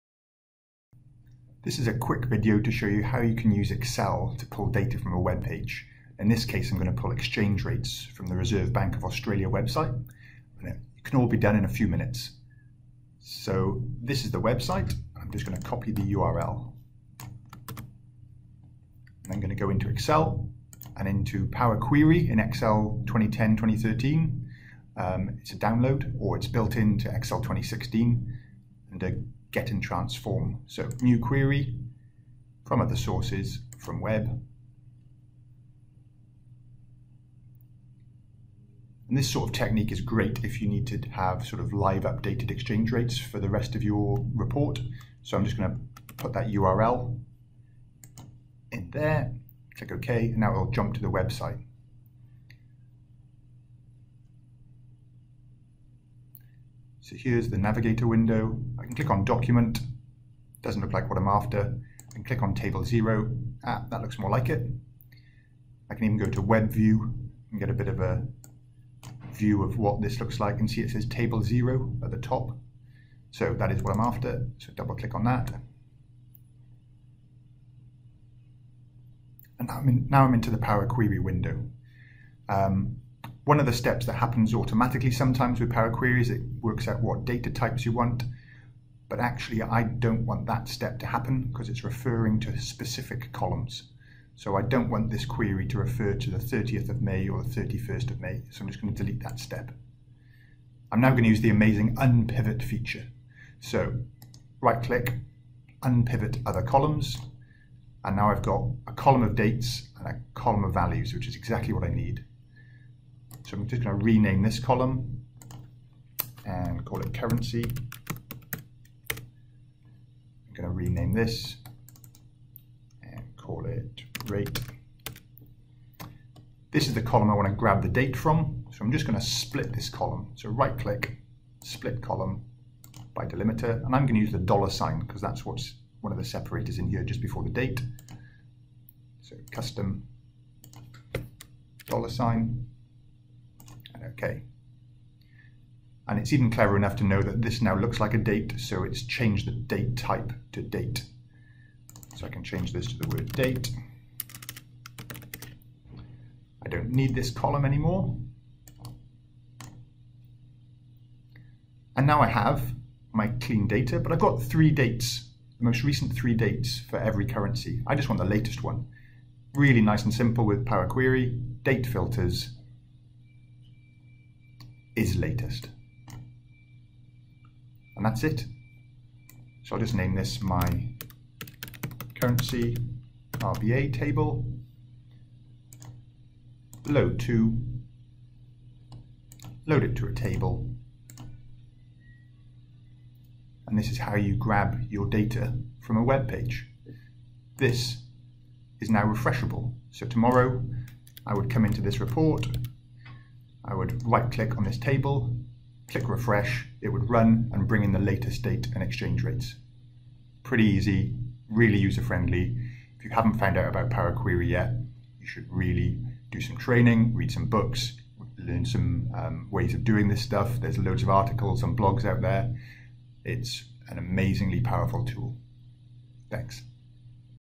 How to pull Data From Web into Excel using Power Query
Using Power Query to get data from a website